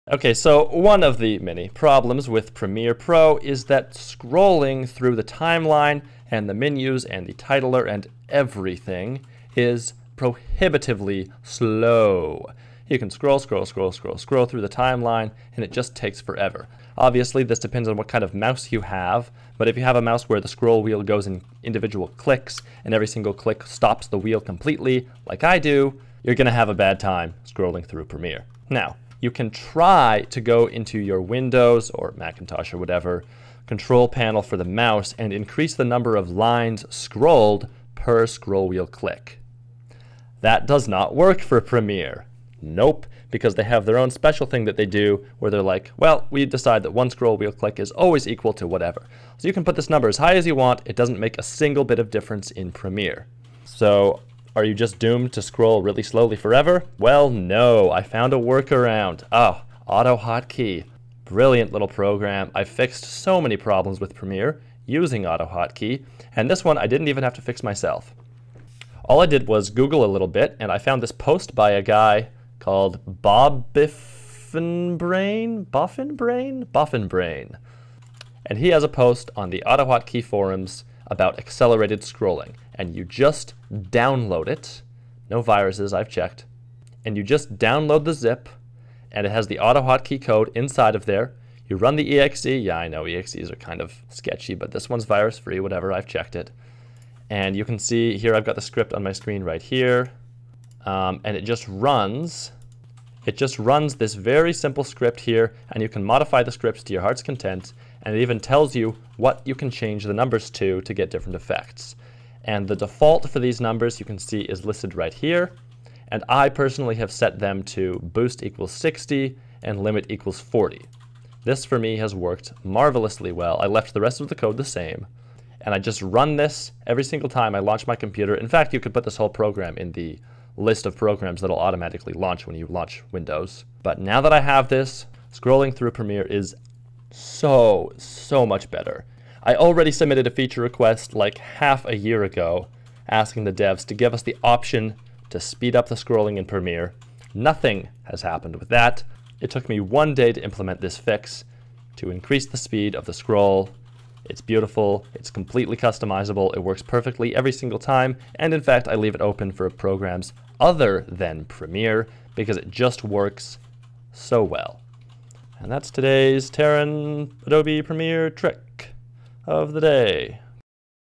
How to enable FASTER SCROLLING in Premiere Pro -- and everywhere else!
TL;DW?

Also, you can add a shortcut to it to your start folder, so that it will launch when you reboot!
Startup folder:
C:\Users\[your_username_here]\AppData\Roaming\Microsoft\Windows\Start Menu\Programs\Startup
Or you can use this one instead, but it affects all users:
C:\ProgramData\Microsoft\Windows\Start Menu\Programs\StartUp

Below is the original source, but the .exe brings up some warnings on Malwarebytes, so you should just avoid it, and just download autohotkey and run the code that way: